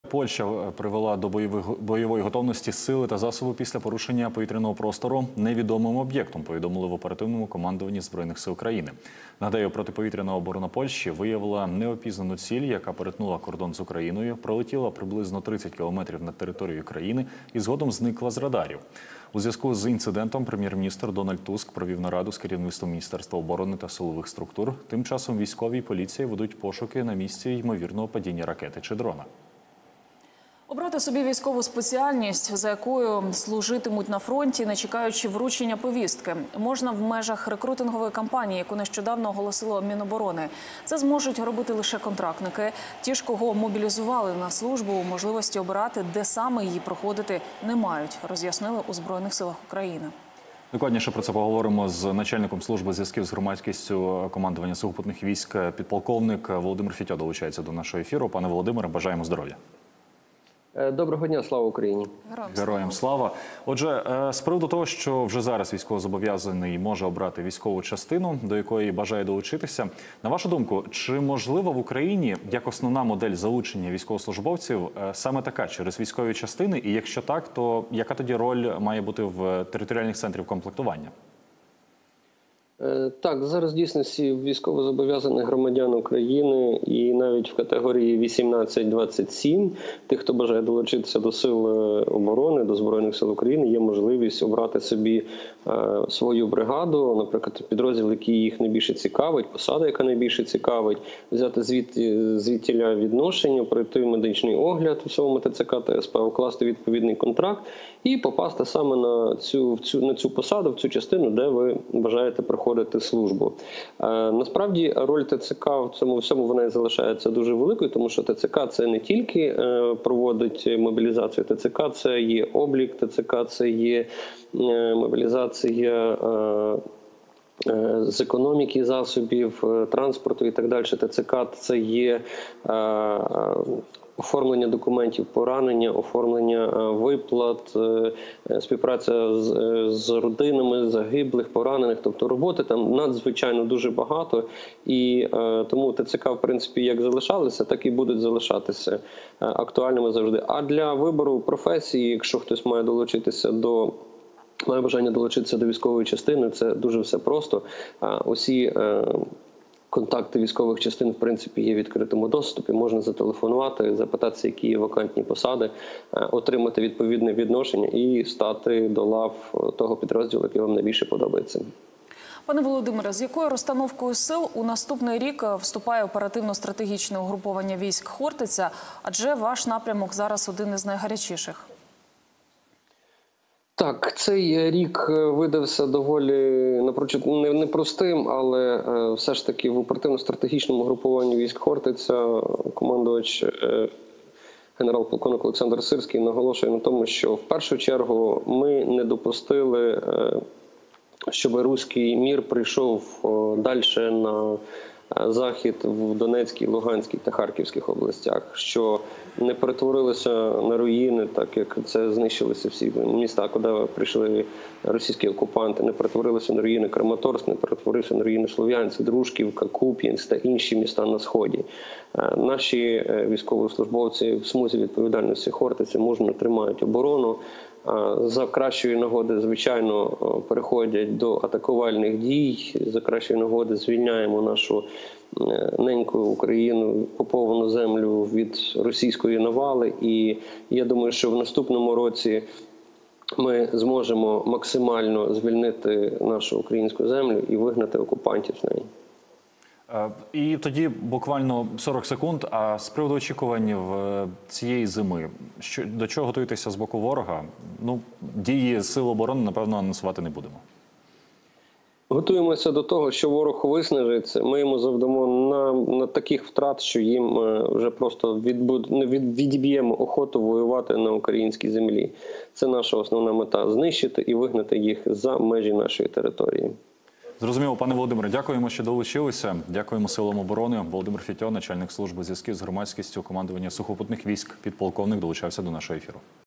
Такі ВТРАТИ Росії і не снилися. ЗСУ готують СЮРПРИЗИ для окупантів. Тотальне виснаження РФ. ФІТЬО
Підполковник, начальник служби зв'язків з командування сухопутних військ ЗСУ громадськістю Володимир Фітьо про рекрутинг у ЗСУ та плани на 2024 рік.